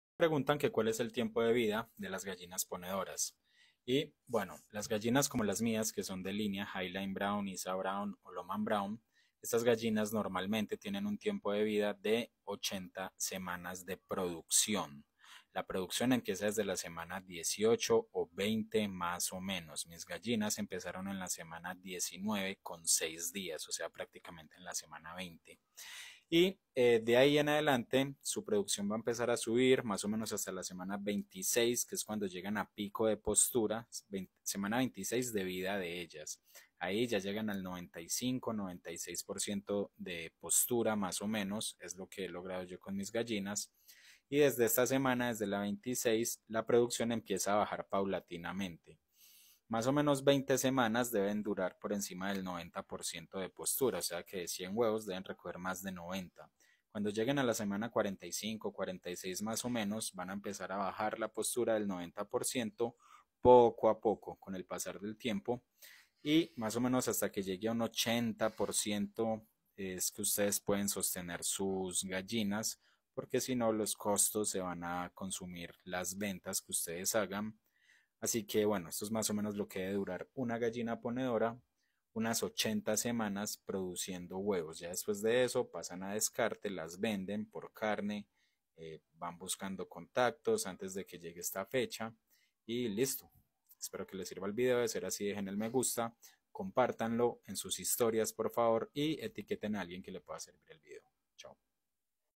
¿Cuál es el tiempo de vida de las gallinas ponedoras?
En el video de hoy exploramos uno de los aspectos más interesantes en la vida de las gallinas ponedoras: su esperanza de vida. Descubre cuánto tiempo viven estas aves y qué factores influyen en su longevidad.

 Analizamos desde la genética y la raza hasta el cuidado y el ambiente en el que se crían. Profundizamos en cómo la nutrición, el manejo del estrés y la prevención de enfermedades pueden extender la vida de estas aves. Además, contrastamos la vida útil de las gallinas en producción con su vida total, proporcionando una perspectiva integral sobre su ciclo de vida completo.

 Este video es una guía valiosa tanto para criadores experimentados como para aquellos que recién comienzan en el mundo de la avicultura, ofreciendo conocimientos clave para el cuidado y bienestar de las gallinas ponedoras. Acompáñanos en este viaje informativo para comprender mejor la vida de estas increíbles aves productoras.